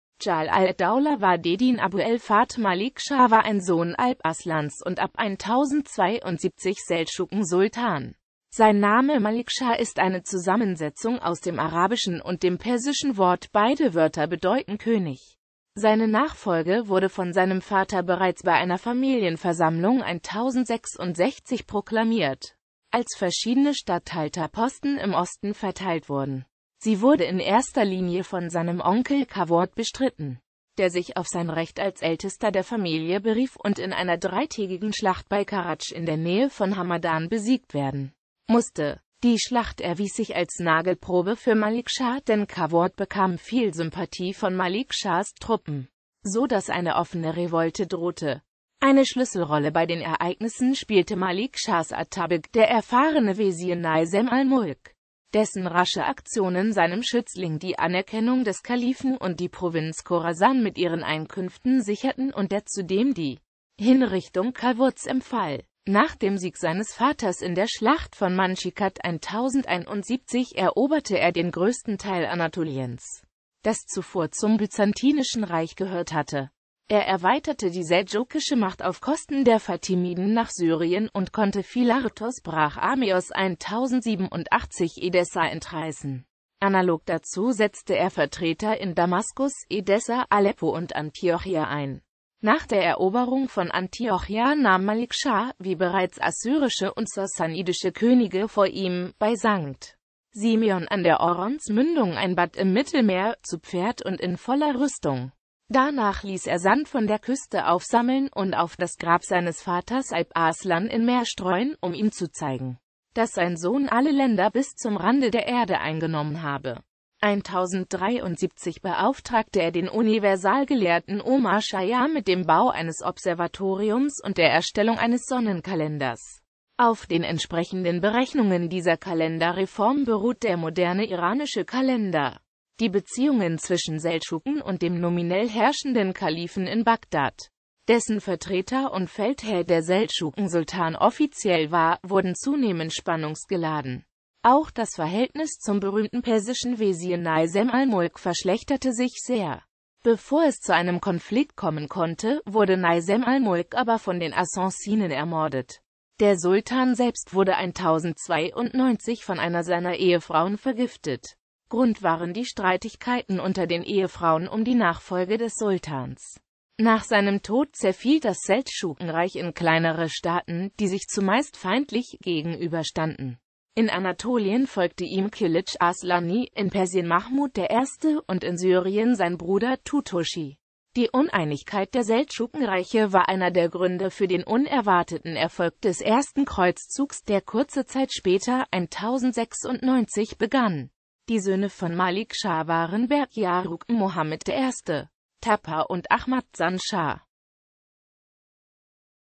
Malik Schah I.
Vielen Dank für Ihre Unterstützung:

Dschalal ad-Daula wa-d-Din Abu l-Fath Malik-Schah war ein Sohn Alp Arslans und ab 1072 Seldschuken-Sultan.Sein Name Malik-Schah ist eine Zusammensetzung aus dem arabischen und dem persischen Wort ; beide Wörter bedeuten König.Seine Nachfolge wurde von seinem Vater bereits bei einer Familienversammlung 1066 proklamiert, als verschiedene Statthalterposten im Osten verteilt wurden.Sie wurde in erster Linie von seinem Onkel Qawurd bestritten, der sich auf sein Recht als Ältester der Familie berief und in einer dreitägigen Schlacht bei Karadsch in der Nähe von Hamadan besiegt werden musste.

Bild-Copyright-Informationen
Urheber Info: Bilinmiyor, anonim KM